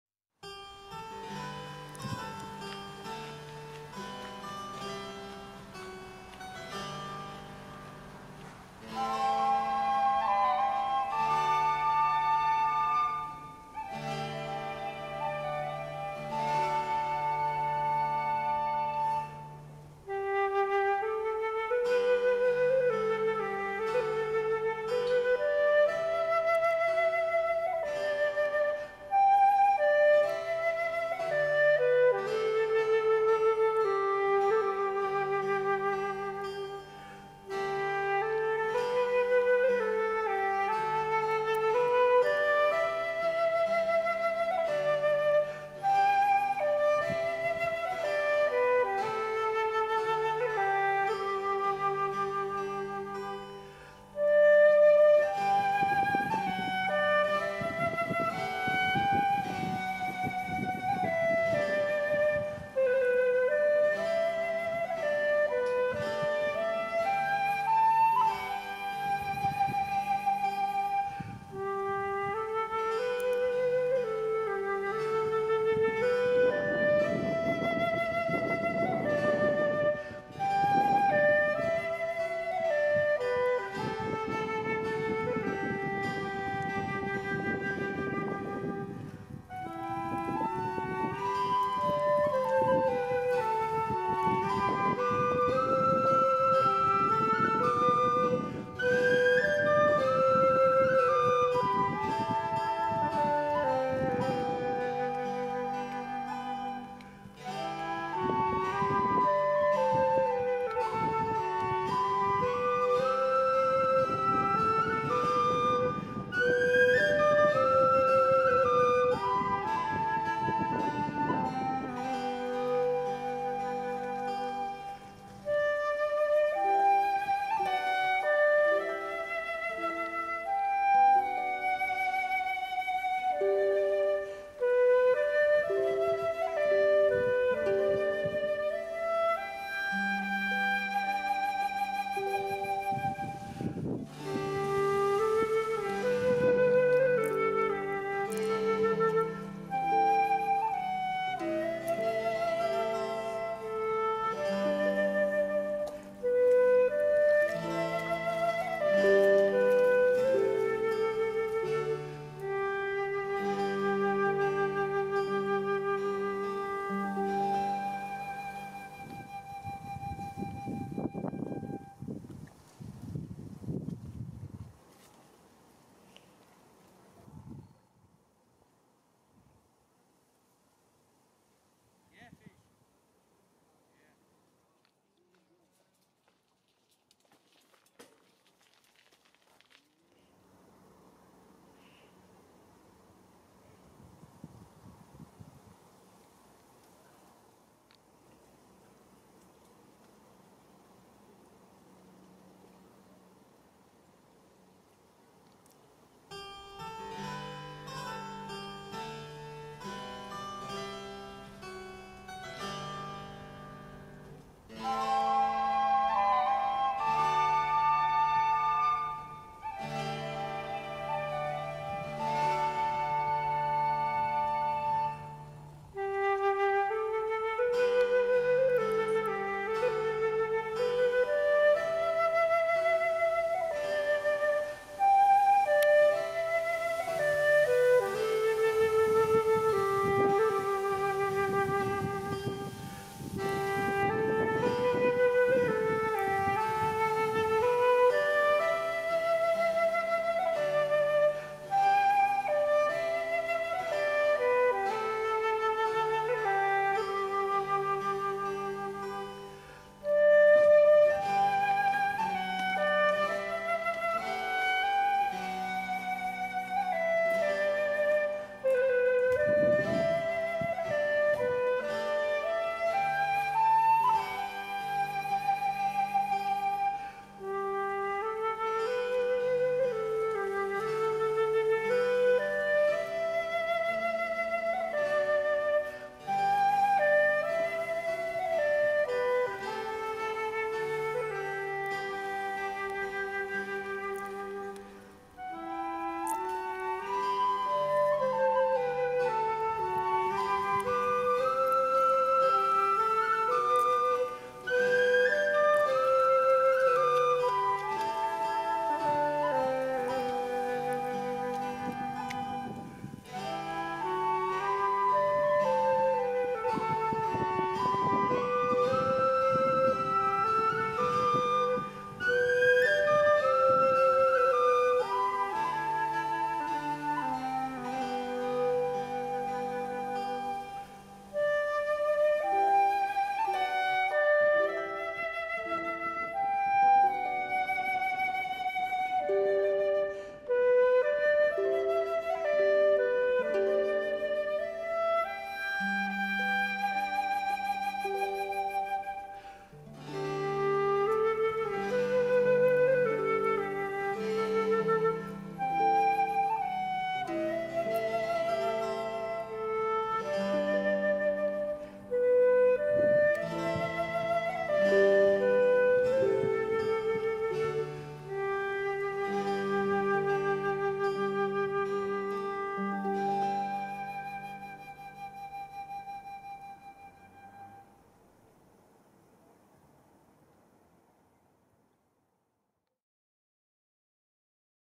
2023-08-14 Ellie's Walk - Doorly Park
Summer weather and Ellie the Irish Softcoated Whwaten Terrer enjoys a walk in Sligo's Doorly Park along the River Garavogue ... and in fact uses the River for a drink and to cool her feet.